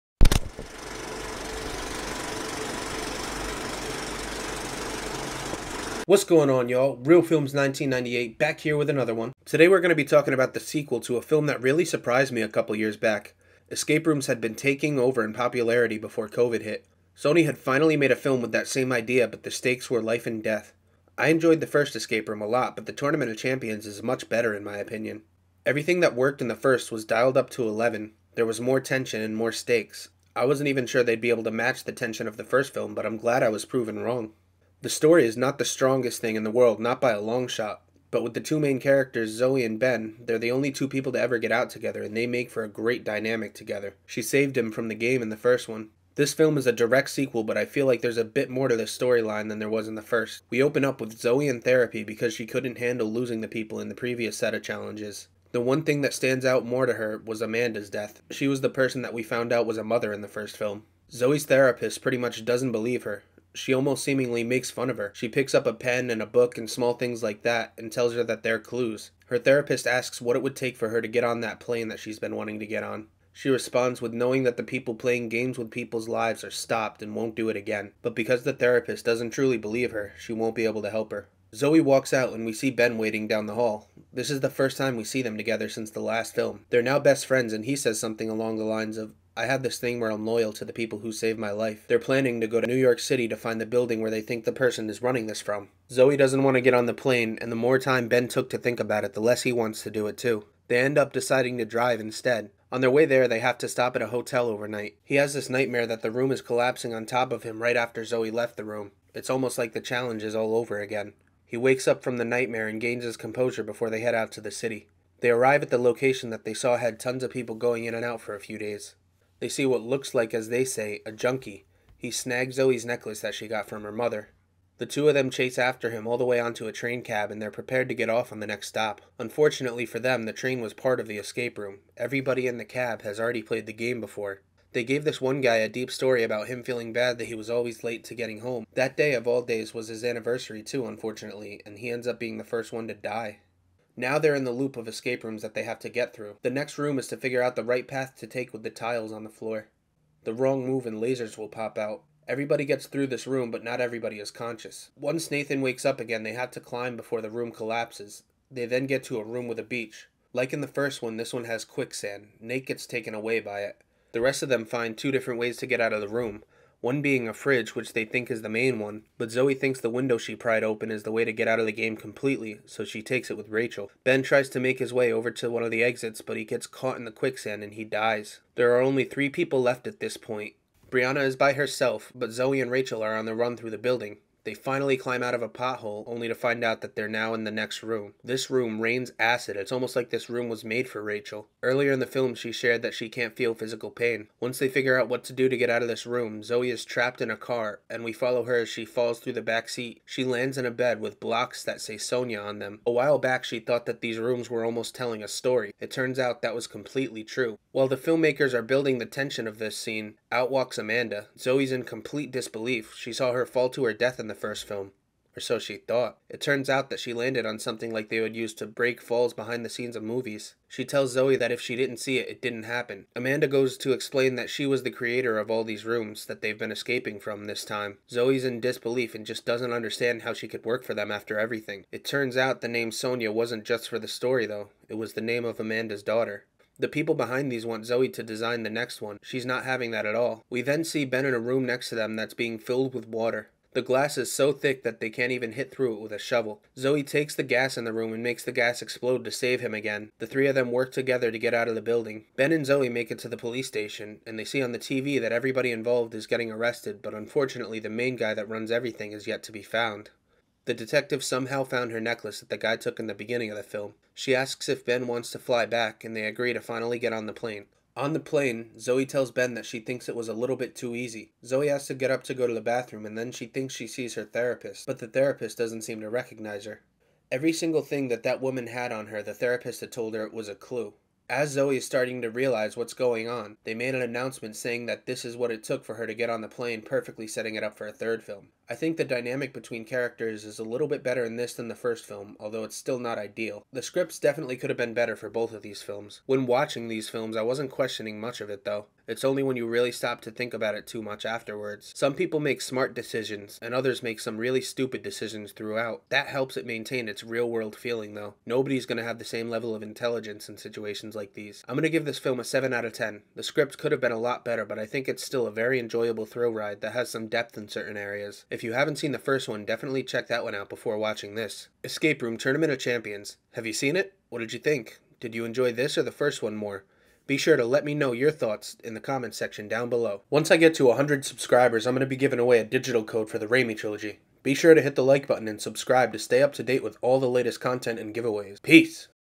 Escape Room Tournament of Champions Review (Spoilers)
I Review the sequel to Sony's 2019 film Escape Room. Escape Room Tournament of Champions has so many fun and tense thrills.